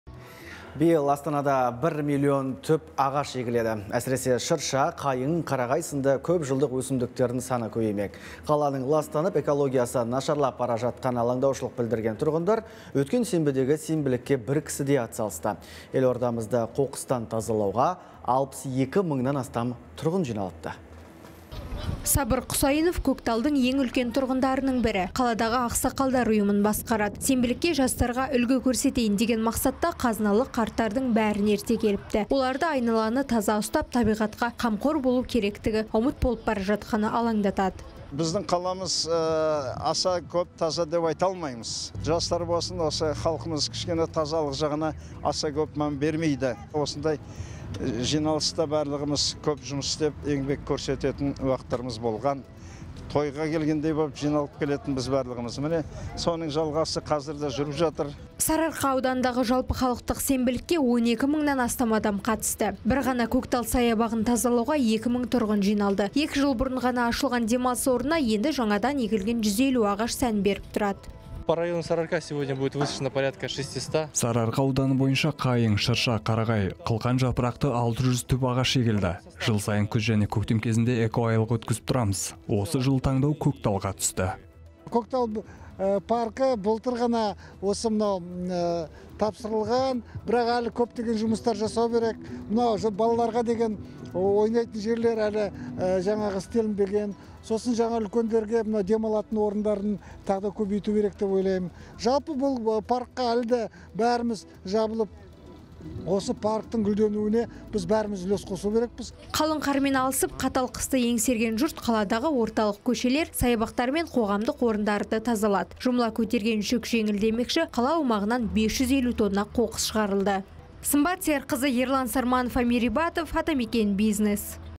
Астанадағы сенбілікке 62 мың тұрғын атсалысты
Биыл Астанада 1 миллион түп ағаш егіледі. Әсіресе шырша, қайың, қарағай сынды көпжылдық өсімдіктердің санын көбеймек. Қаланың ластанып, экологиясы нашарлап бара жатқанына алаңдаушылық білдірген тұрғындар өткен сенбідегі сенбілікке бір кісідей атсалысты. Елордамызды қоқыстан тазалауға 62 мыңнан астам тұрғын жиналыпты